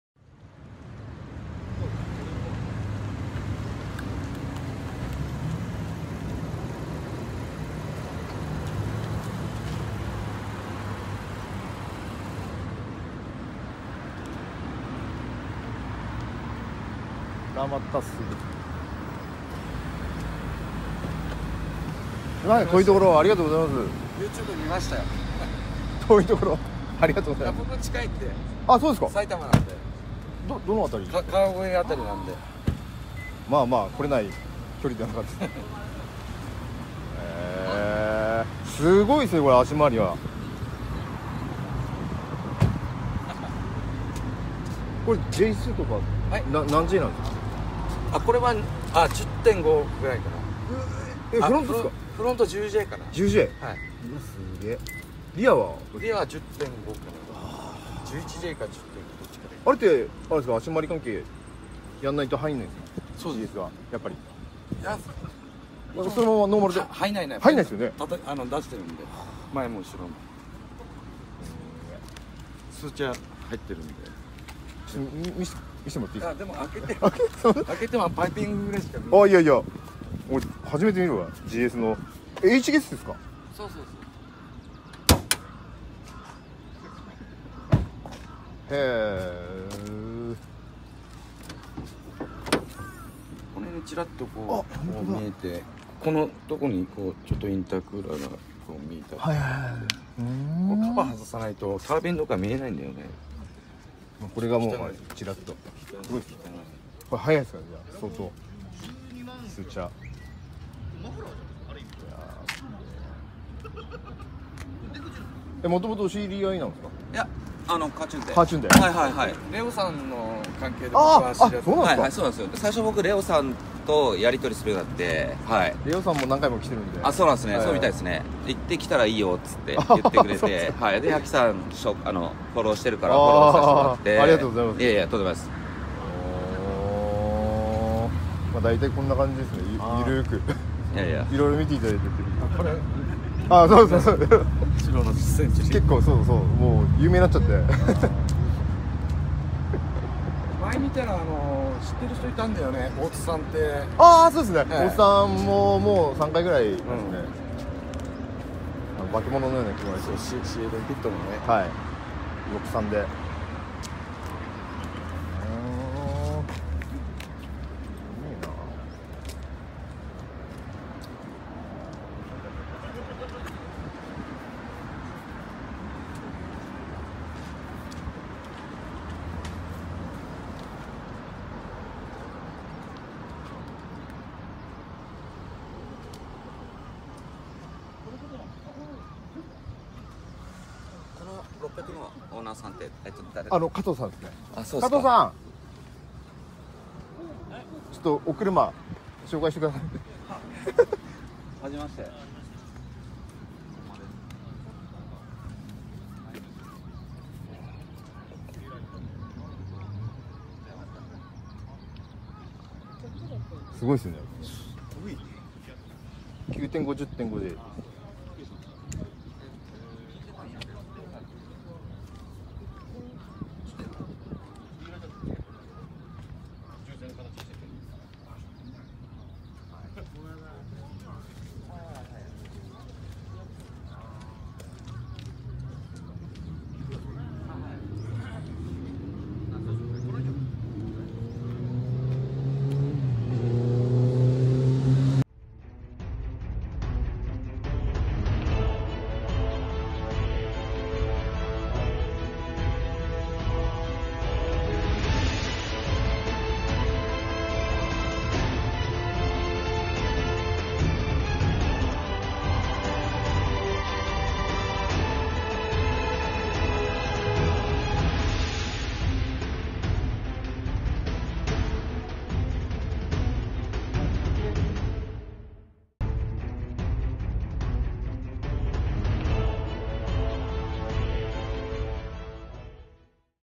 【HKSのスーパーチャージャー搭載のレクサスGS350‼️】RonさんのスーパーGSが初登場‼️【花園MT 2019.4.21】
凄いスペックのレクサスGS350が初参加です！

HKS GTスーパーチャージャー
escalation オリジナルマフラー
HKSインタークーラー
HKSオイルクーラー

めちゃくちゃ速そう！！
今度、隣乗せてください！